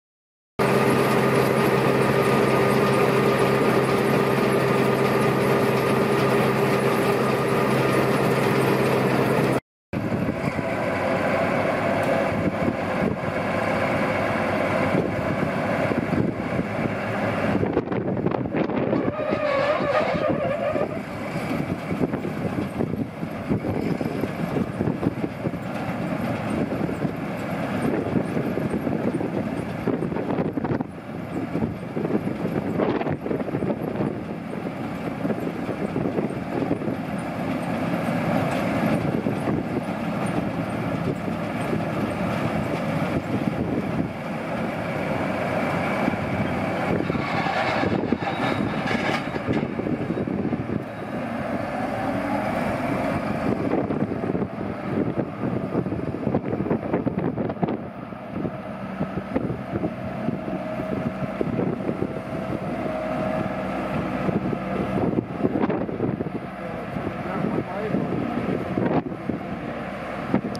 John Deere 4420 Combine changeover Oats to Corn, Vintage Analog Stewart Warner Tachometer
Checking shaft speed with a vintage analog Stewart Warner tachometer on John Deere 4420 combine in preparation for corn harvest